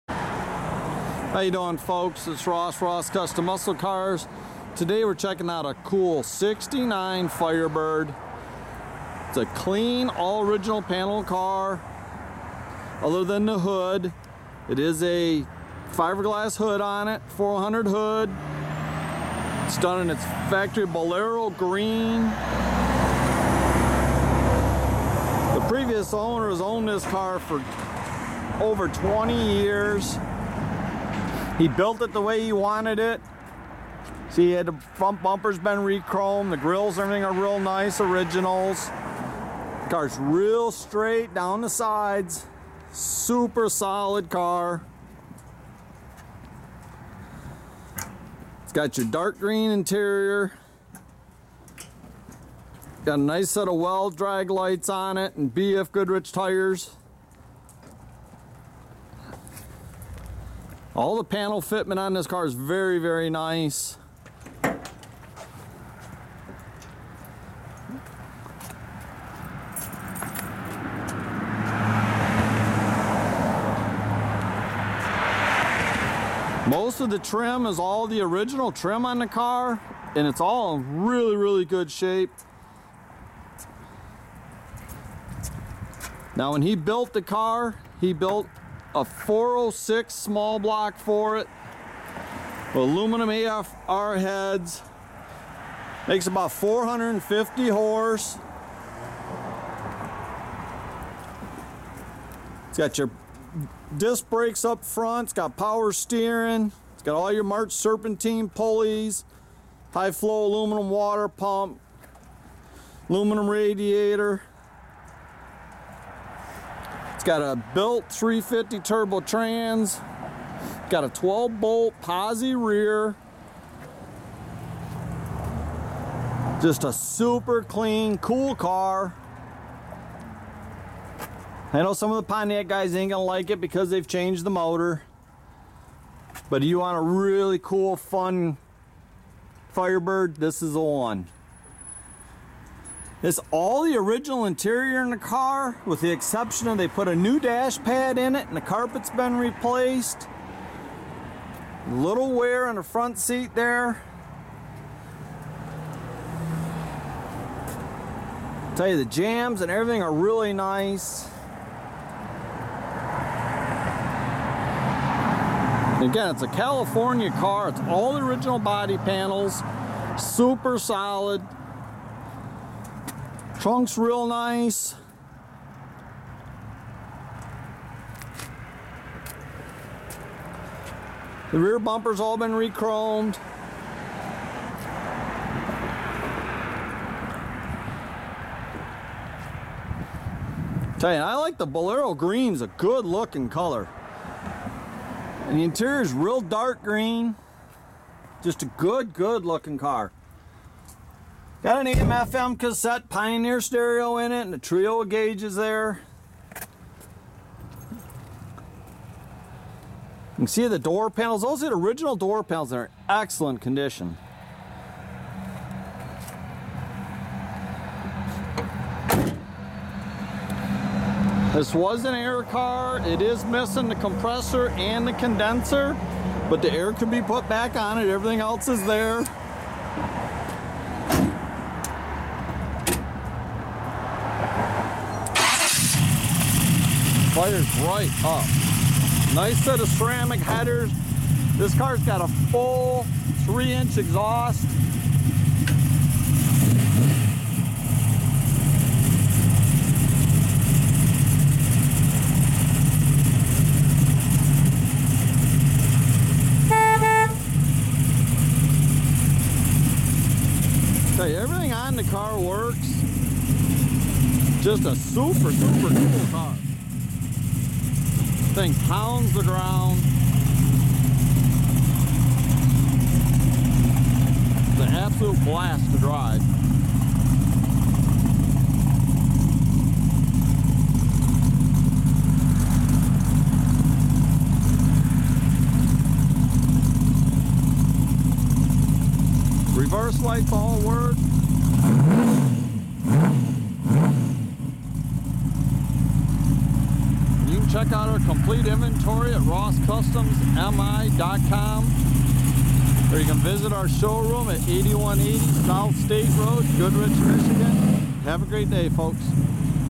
RossCustomsMI.com - SOLD SOLD - 1969 Pontiac Firebird
This is a clean all original panel California built Firebird that has had a recent body up restoration done in a very nice Verdoro Green base coat clear coat, fiberglass 400 hood, new window felts and door sills, new side view mirror, all stainless moldings are original in very good condition, door handles have also been replaced, re-chromed front bumper, headlight bezels are original in excellent condition, new wheel well moldings, rocker moldings look like new, re-chromed rear bumper.

Factory dark green interior, new dash pad, original instrument cluster and dash bezel in excellent condition, Pioneer AM/FM/Cassette stereo system, also has a trio of gauges nicely mounted under dash, AutoMeter tach with shift light mounted on column, headliner is original in excellent condition, original front and rear door panels in excellent condition, front and rear seat covers are also original in very good condition - with exception of drivers seat has some wear (front covers have been ordered), nicely outfitted with B & M ratchet shifter, front and rear seat belts with shoulder straps, new carpet, original sill plates.

Under the hood the original motor has been replaced with a professionally built 406 small block Chevy, Eagle crank and rods, forged pistons, Comp cam, AFR aluminum cylinder heads, roller rockers, GM Performance bow tie aluminum intake, Holley 750 dual feed carb, MSD ignition with billet MSD distributor, 6AL box, billet aluminum valve covers, K & N breather, HiFlow aluminum water pump, March serpentine pulley kit, power steering, heavy duty radiator with electric fans, CPP front disc brakes, this strong running 406 is backed up with a 350 turbo trans, B & M shift kit, 3000 stall converter, Hooker ceramic coated long tub headers into 3 inch custom exhaust out the back in stock location with Edelbrock performance mufflers, 12 bolt posi rear end with 390 gear, sub-frame connectors and slapper bars.

Rides on a set of Weld Draglites 15x6 up front 215/70/15 and 15x8 on the rear 275/60/15 with BF Goodrich TA Radials all around.

This Firebird is ready to cruise and enjoy.